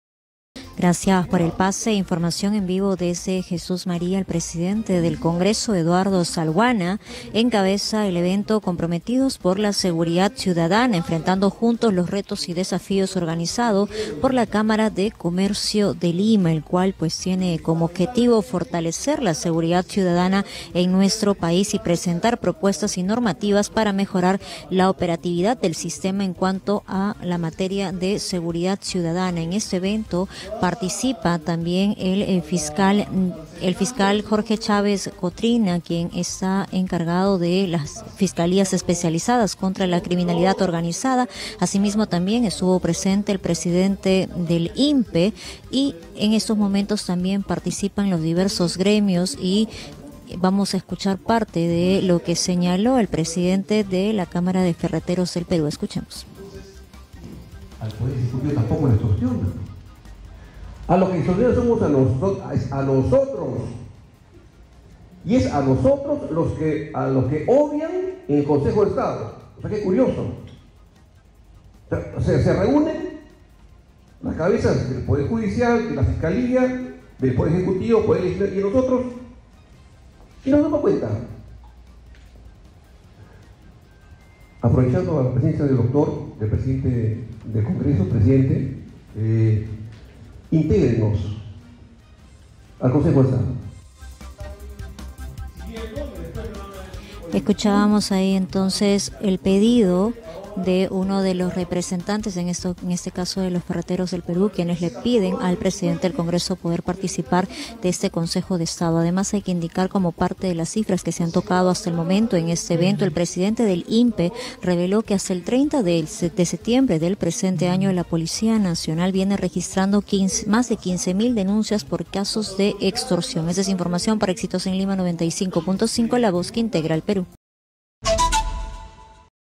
🔴🔵"Nos obvian": Ferreteros piden al Congreso ser incluidos en Consejo de Estado ante ola de crímenes
Tras la convocatoria del Consejo de Estado ante crisis de inseguridad, ferreteros pidieron ser incluidos en dicha reunión: "¿Y nosotros? Intégrenos", indicaron al presidente del Congreso, Eduardo Salhuana.

Exitosa Perú
"Exitosa: La voz que integra al Perú"

Corporación Universal